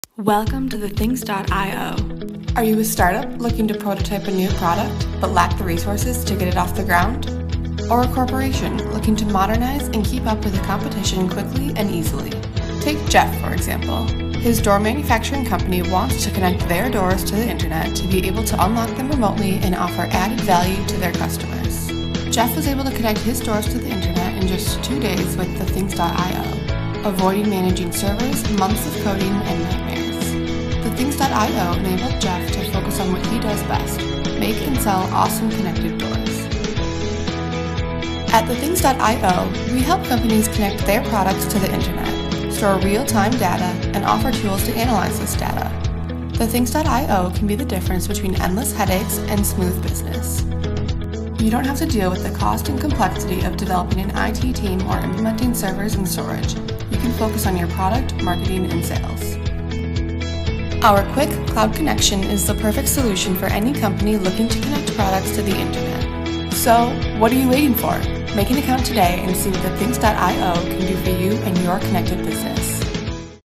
thethings.iO - the simplest IoT platform
thethings.iO is the most simple Internet of Things platform in the market.

thethings.iO is the IoT platform that enables fast and scalable connection of things to the Internet, allowing you to monitor and manage your devices in real time and get flexible analytic reports.

If your IoT needs to store real-time data, process big data and visualize analytics in a dashboard, meet thethings.iO! We are your perfect solution!